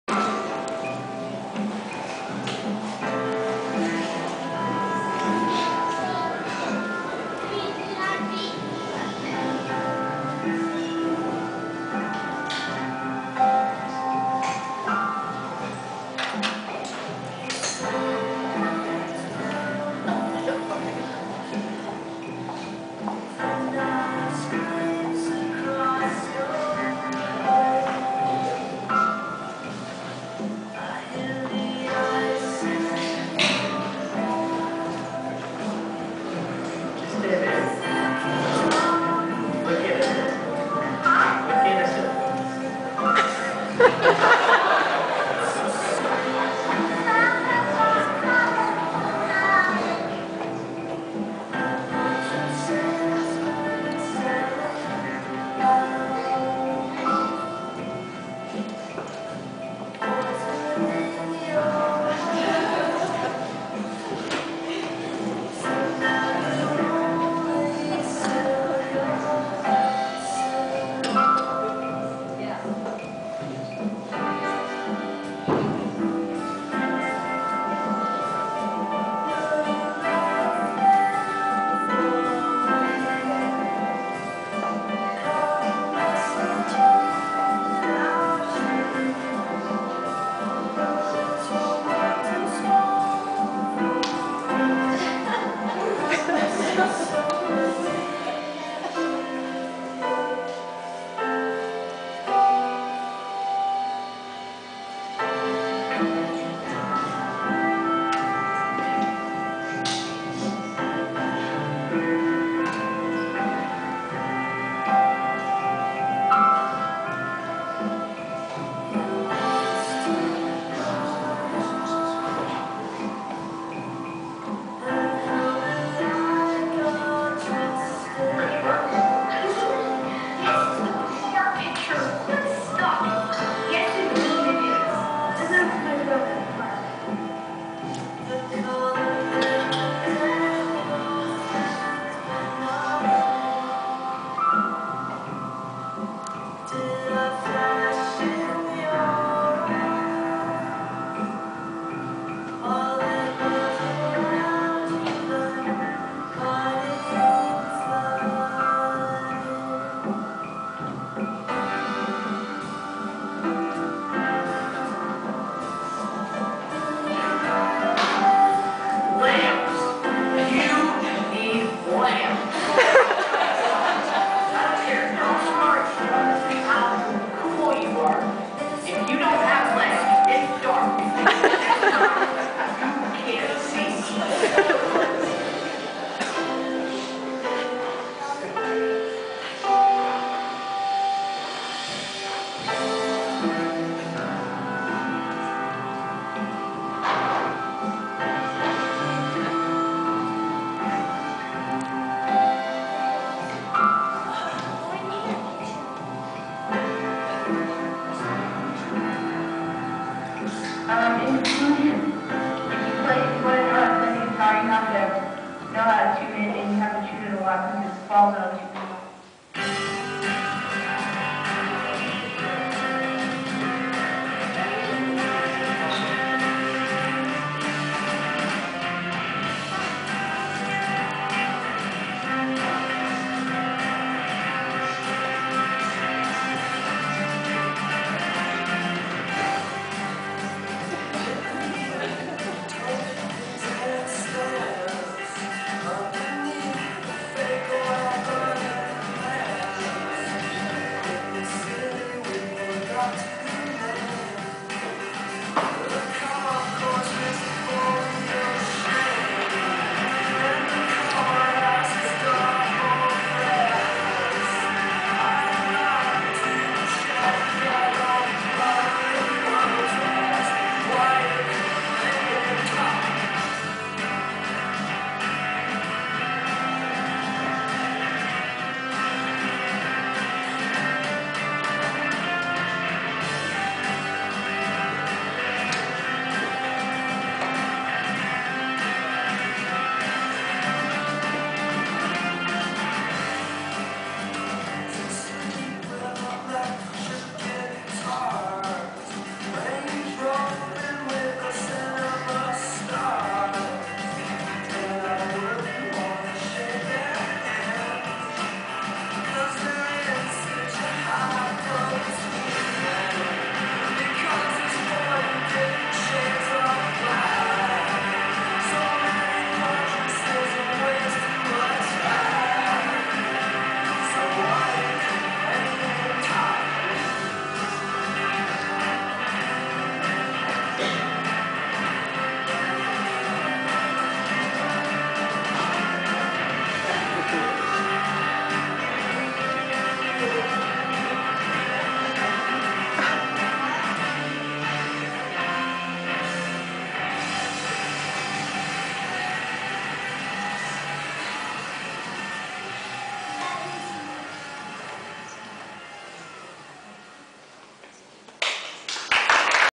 Mark Mercer slide show
Slide show presented at reception after memorial service.